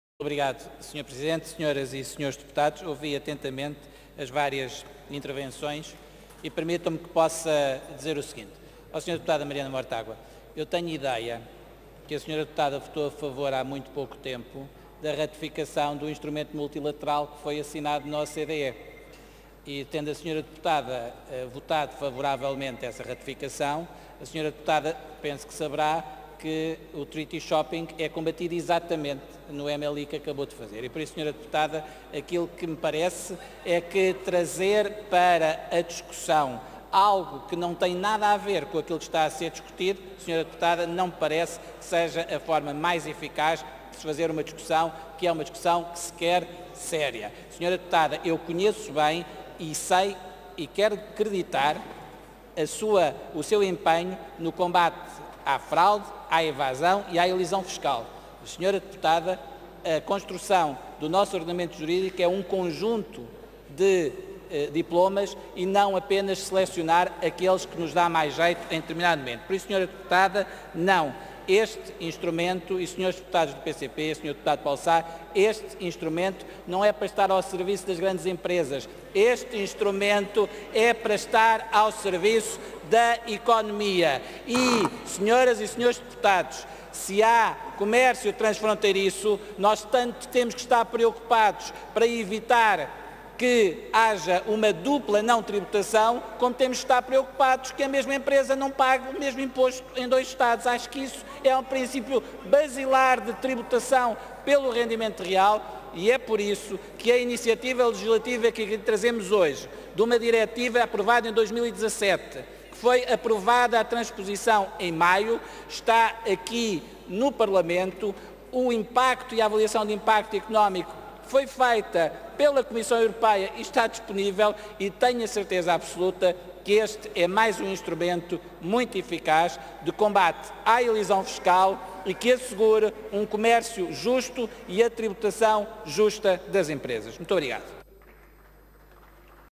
04-07-2019 - Debate | Resolução de litígios, dupla tributação | António Mendonça Mendes 2ª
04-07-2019 | 2ª Intervenção do Secretário de Estado dos Assuntos Fiscais António Mendonça Mendes, no âmbito da Proposta de Lei (GOV) Resolução de litígios que envolvam as autoridades de Portugal e de outros Estados-Membros da UE